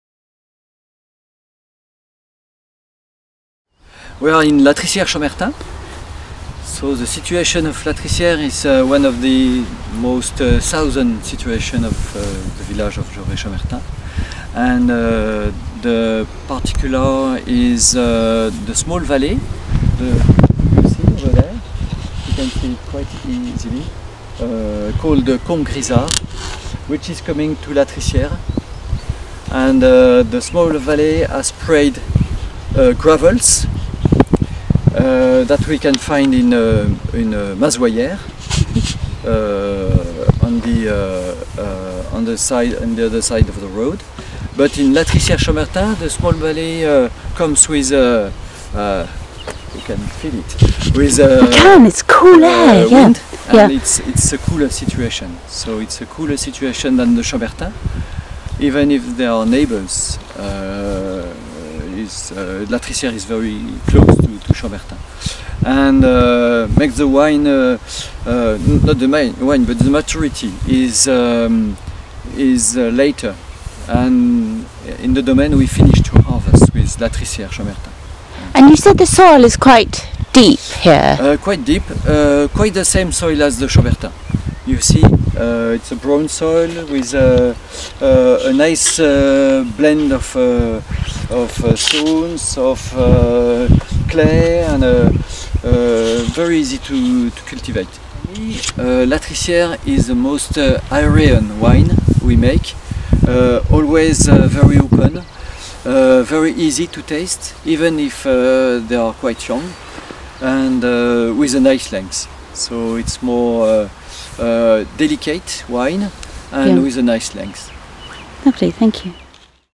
Domaine Rossignol Trapet, Latricières
Domaine Rossignol-Trapet, Latricières Chambertin

Nicolas Rossignol explains this grand cru which lies on the southern side of the village. In the film it’s possible to appreciate the fact it is in a valley - Combe Grisard. Gravels came down this valley - the gravels found across the road in Mazoyères, but it also funnels cold air from the Hautes Côtes to the vineyards below and Latricières lies in its path. The colder air was quite noticeable when filming. So it’s a much cooler situation than neighbouring Chambertin. The fruit ripens later and Nicolas and David Rossignol finish their harvest here. The soil is very like Chambertin with a good mix of stones and clay, quite deep, and very easy to cultivate. In contrast with Chambertin, Nicolas considers Latricières to be the most ‘airy’ wine they make. “Always very open and easy to taste, even when young. A delicate wine with very nice length.”